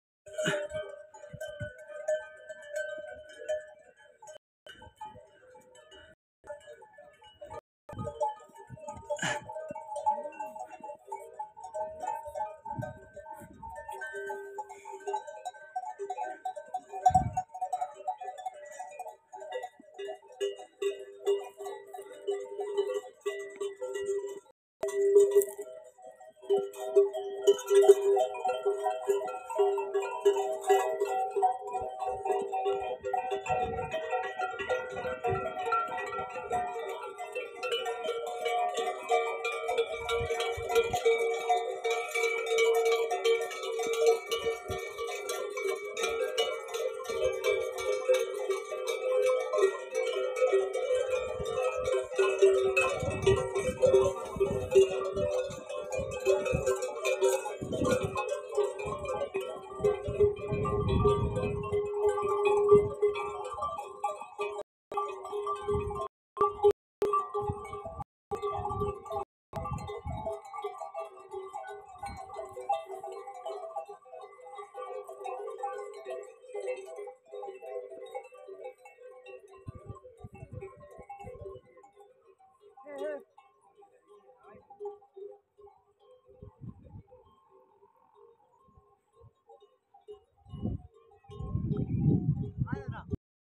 مجموعة من الإبل الصحراوية متجهة إلى الغابة من القرية | فيديو الإبل | الإبل تمشي
حمر النعم على شاطئ صلاله
The beautiful camels on Salalah beach